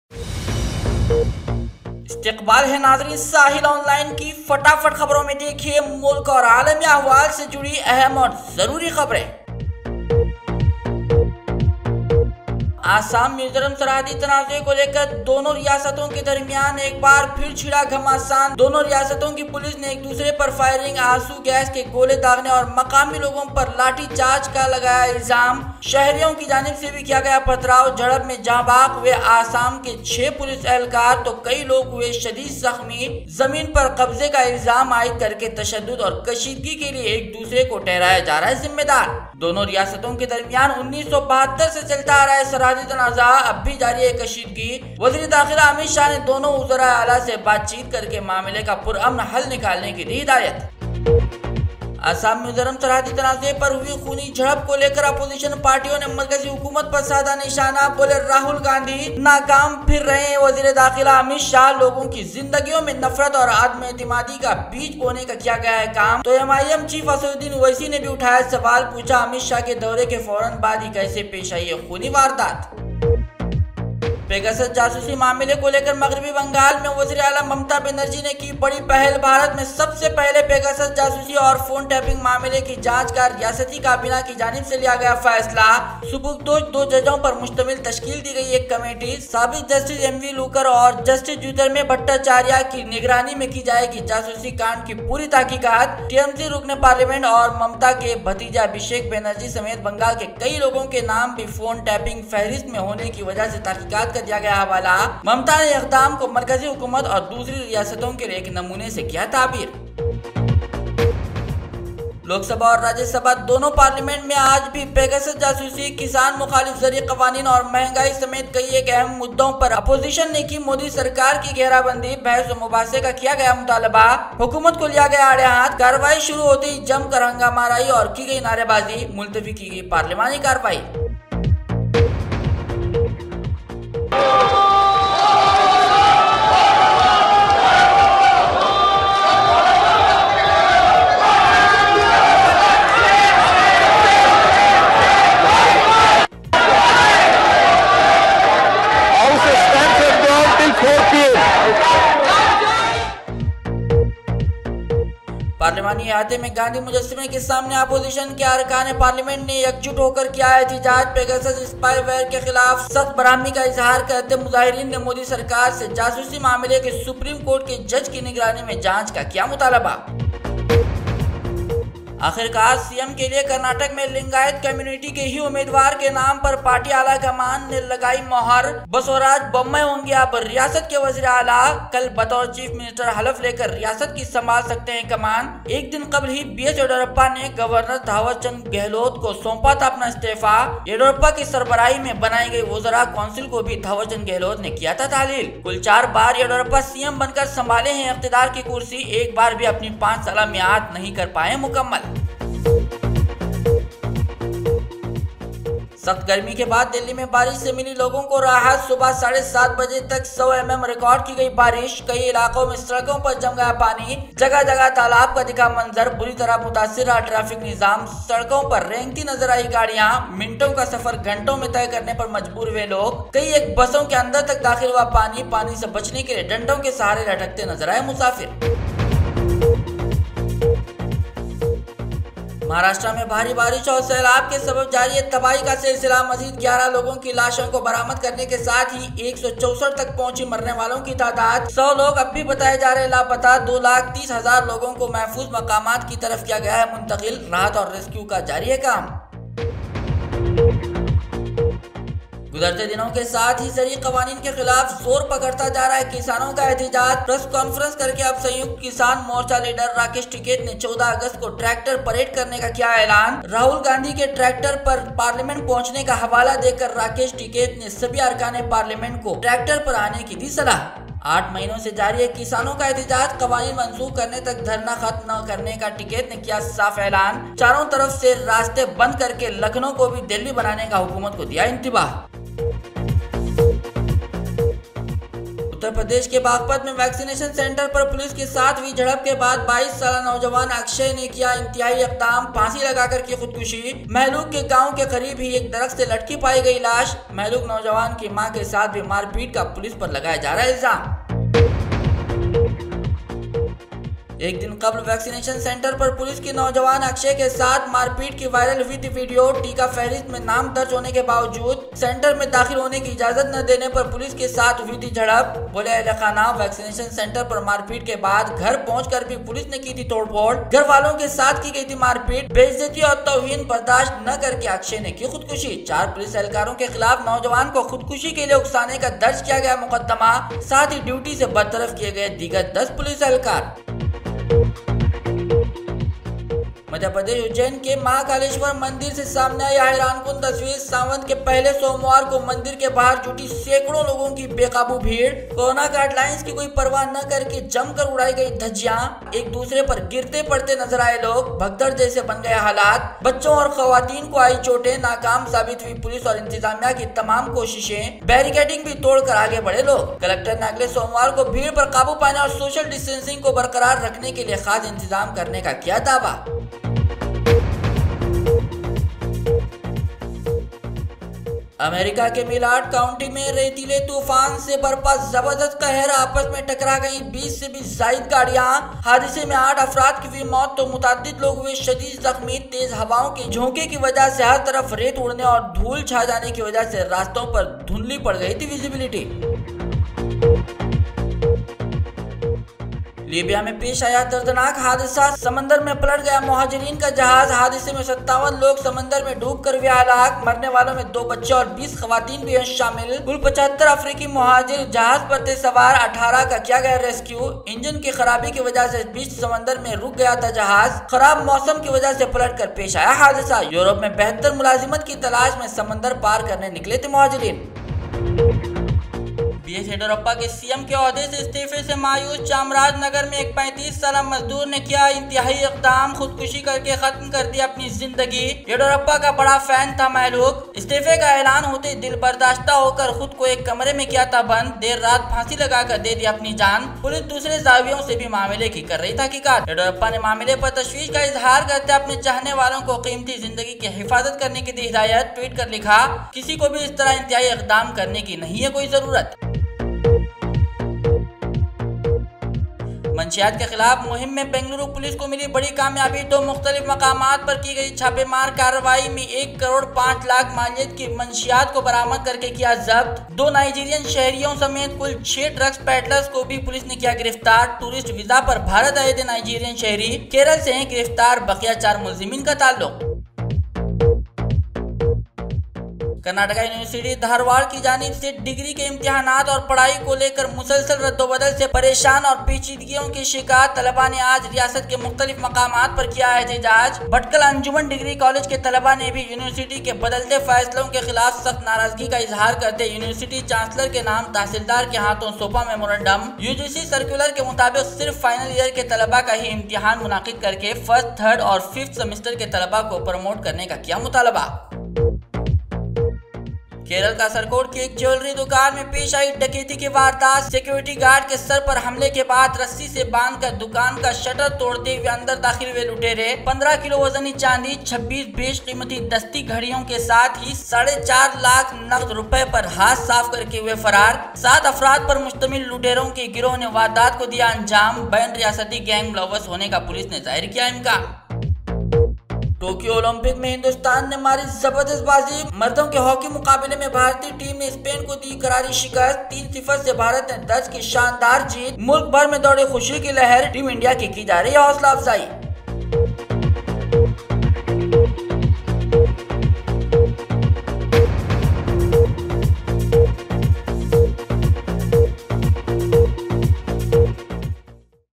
Basavaraj Bommai new Karnataka CM | Assam-Mizoram border violence | KUD students protest | Pegasus
Basavaraj Bommai new Karnataka CM | Assam-Mizoram border violence | KUD exams circular | Pegasus spyware raw | Farmer protest | BS Yediyurappa fan commit suicide